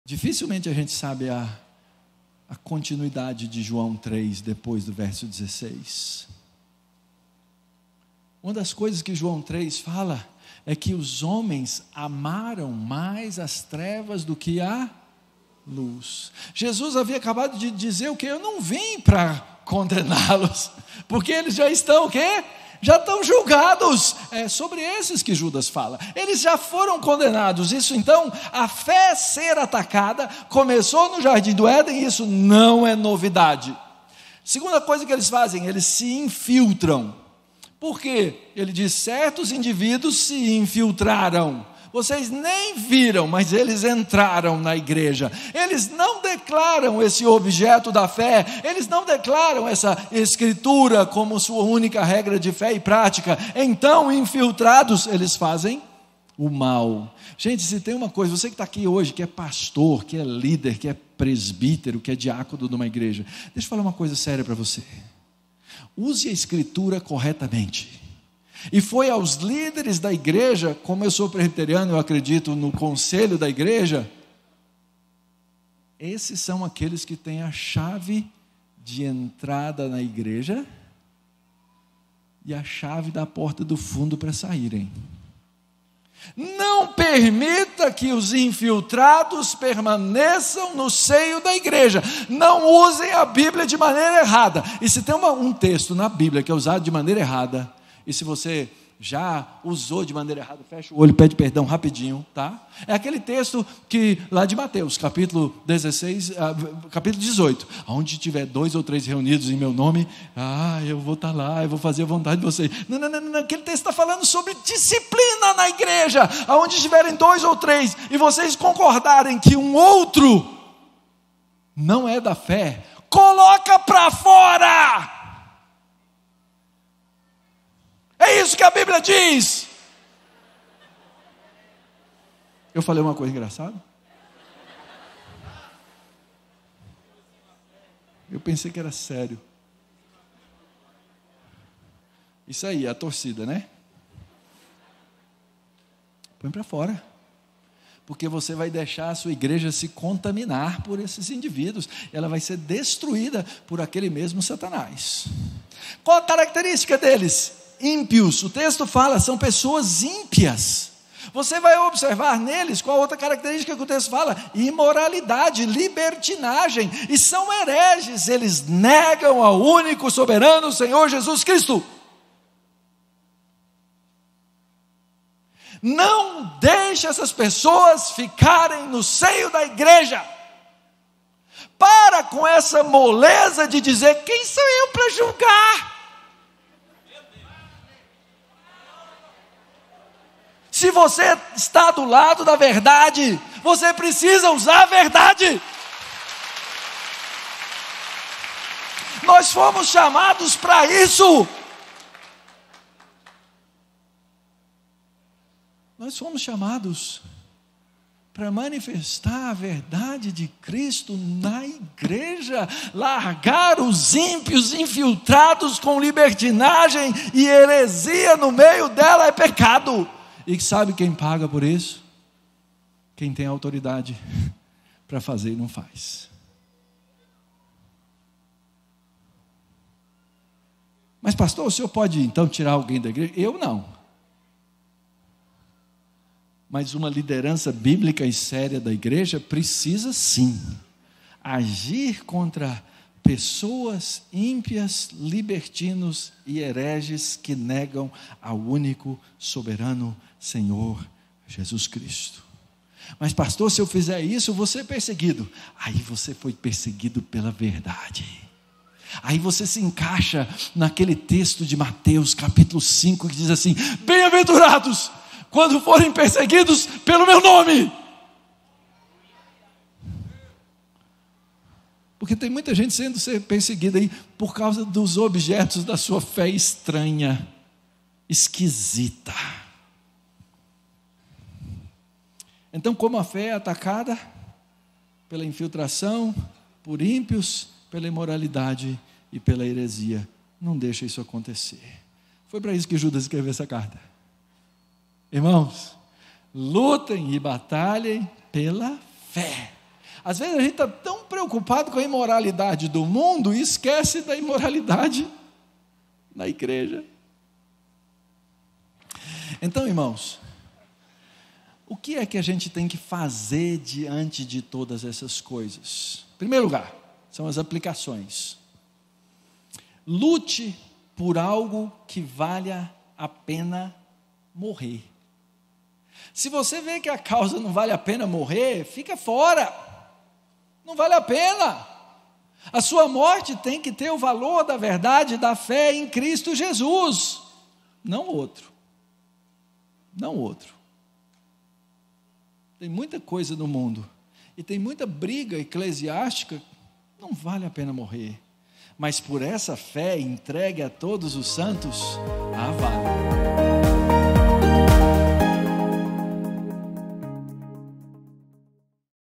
A CAUSA QUE VALE A PENA [ + Mauro Meister ] Confirmado na CC2024
Descubra por que defender a fé vale cada desafio. Neste vídeo, o Pastor Mauro Meister explora como proteger nossa fé contra ataques internos, destacando insights cruciais e a importância de permanecer firme na verdade de Cristo.

Assista agora para fortalecer sua fé e compartilhe com outros que também desejam proteger a fé em Cristo!

Você vai encontrar durante os 6 dias de evento:
--- 15 Plenárias